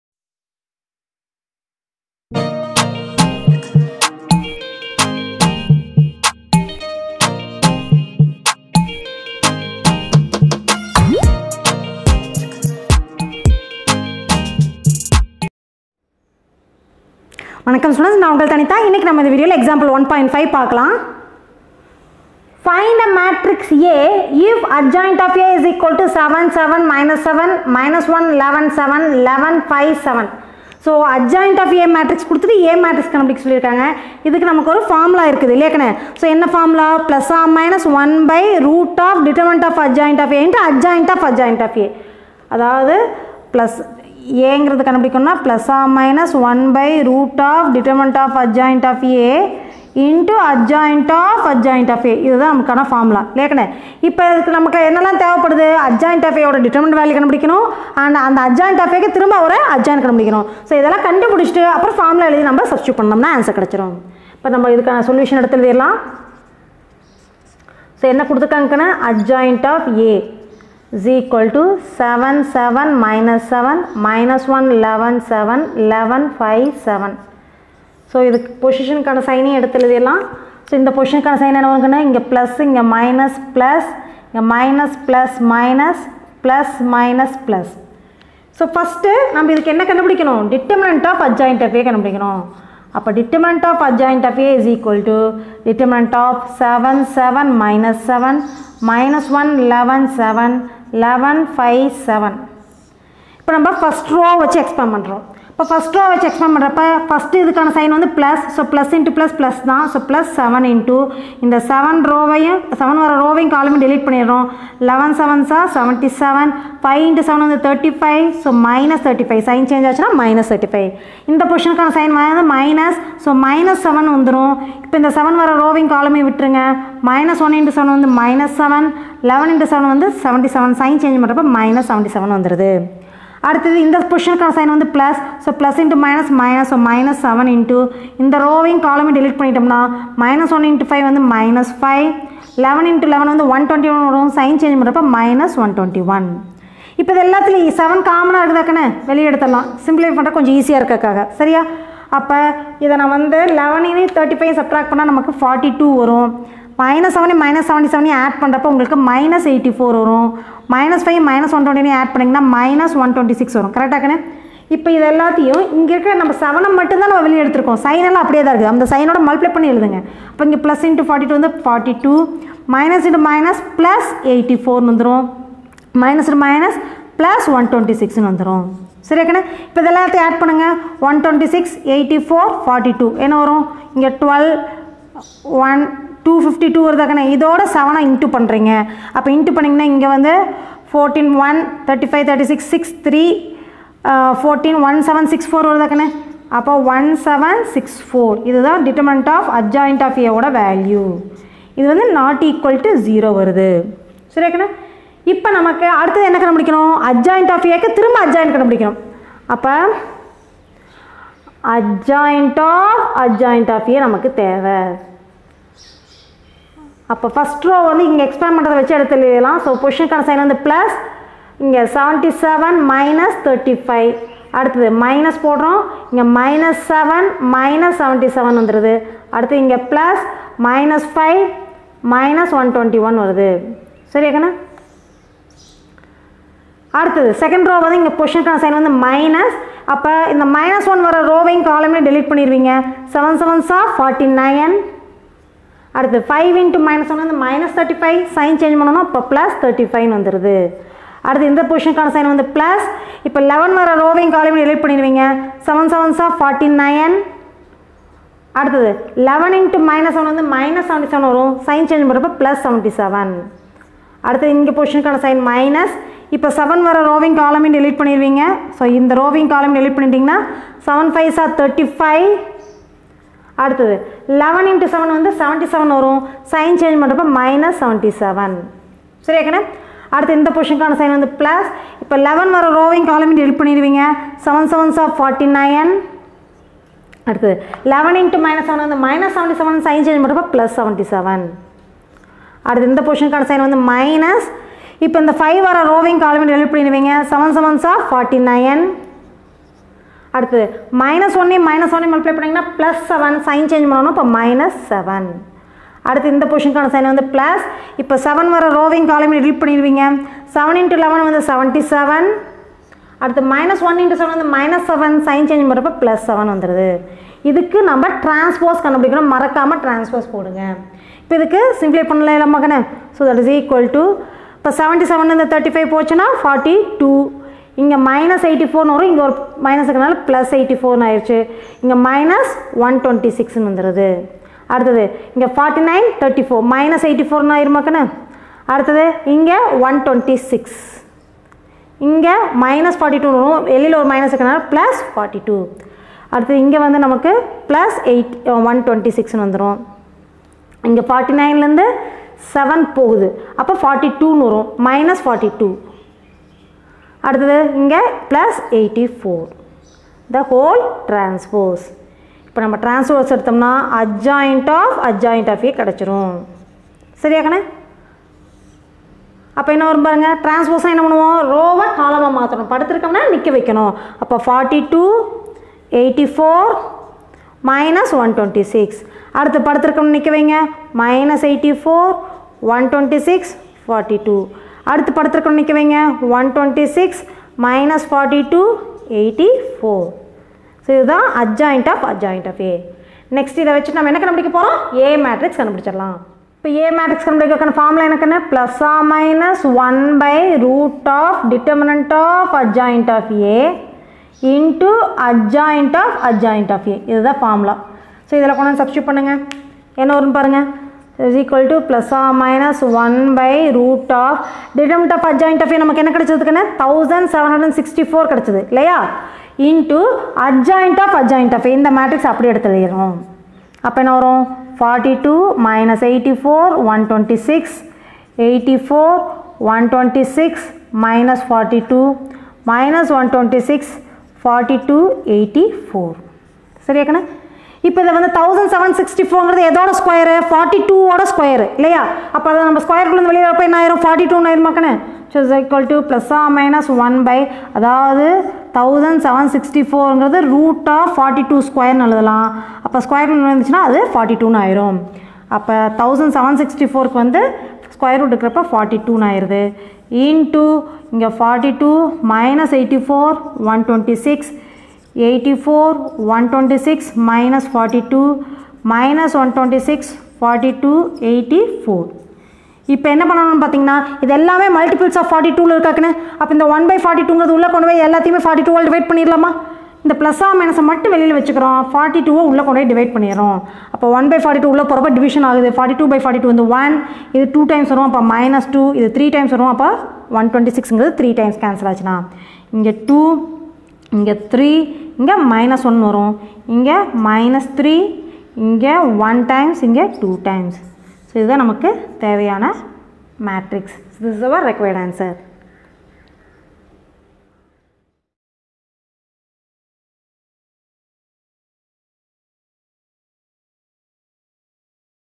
12th Maths, Example : 1.5 in Tamil
12th Maths, Chapter – 1,Example : 1.5 is solved and explained in Tamil